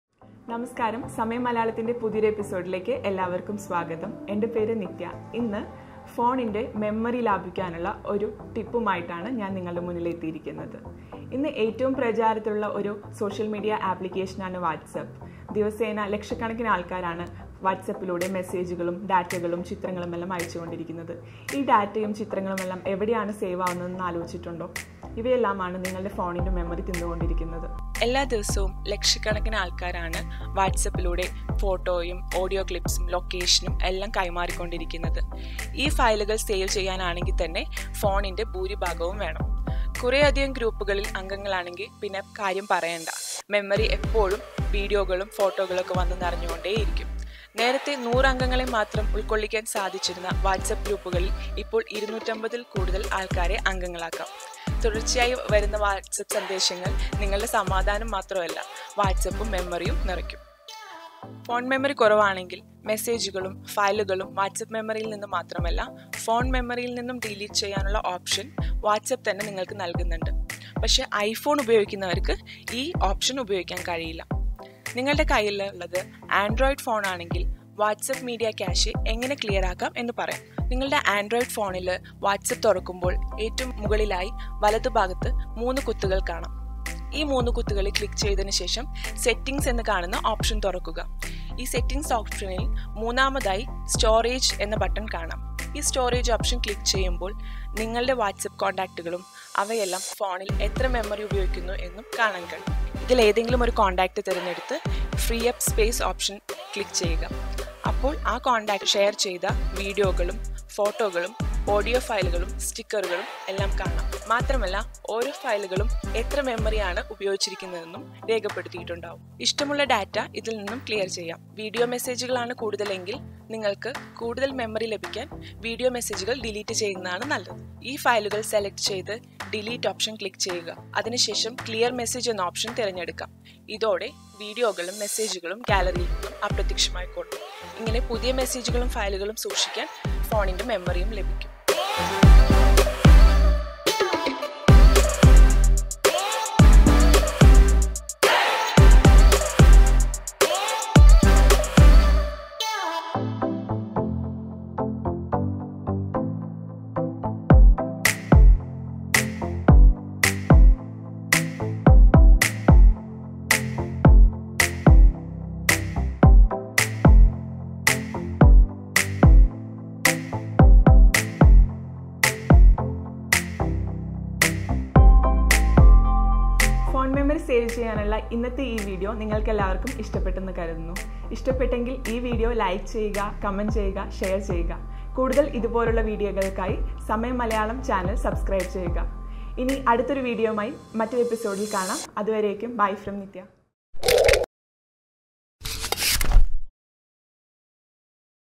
How To Clear Whatsapp Cache? | വാട്സാപ്പ് ക്യാഷെ എങ്ങനെ ഡിലീറ്റ് ചെയ്യാം| Samayam Malayalam
മെസ്സേജുകളും ഫയലുകളും ഫോണിൽ നിറഞ്ഞ് സ്പേസ് കുറയുന്നുണ്ടോ? വാട്സാപ്പ് മെമ്മറിയിൽ നിന്ന് മാത്രമല്ല ഫോൺ മെമ്മറിയിൽ നിന്നും ഫയലുകൾ ഡിലീറ്റ് ചെയ്യാനുള്ള ഓപ്‌ഷൻ വാട്സാപ്പ് നൽകുന്നുണ്ട്.